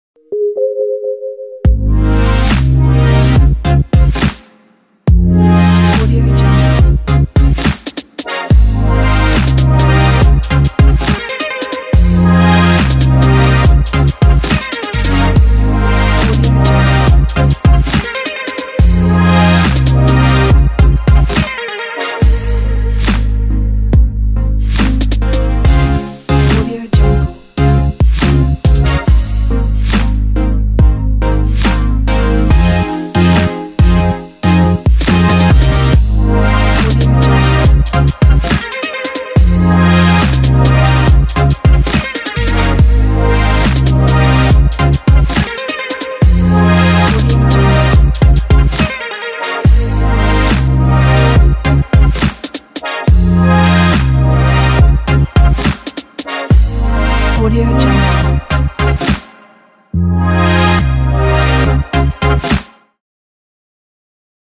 Energetic Opener (After Effects Template)
Features:
- Modular structure
- No plugins required
- Control layer for customization
- Timeline markers for dynamic montage
- Modern design
- Full HD (1920×1080), 30fps (adjustable)
- Multi-purpose
- Works with After Effects CS5.5 and above
- Music and videos are not included